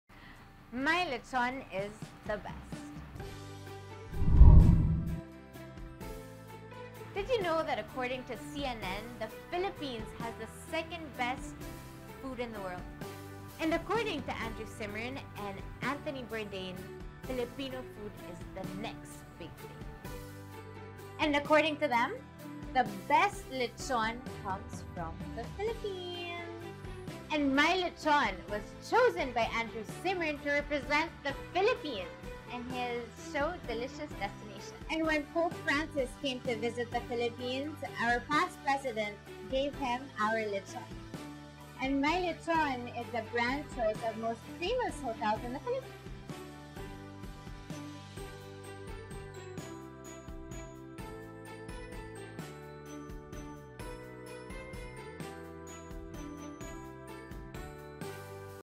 Gwen Zamora's Elarz Pambansang Lechon
Make your celebration complete with the best lechon. FREE BOPIS, DINUGUAN OR PAKLAY when you Download Elarz Pambansang Lechon App for FREE at Google & Apple Store and order whole lechon at Elarz Gwen Zamora Branch!